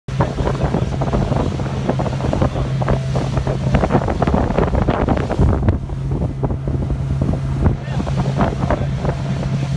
Vacances en Guadeloupe en février 2017
Axelle et Marcello en balade mini croisière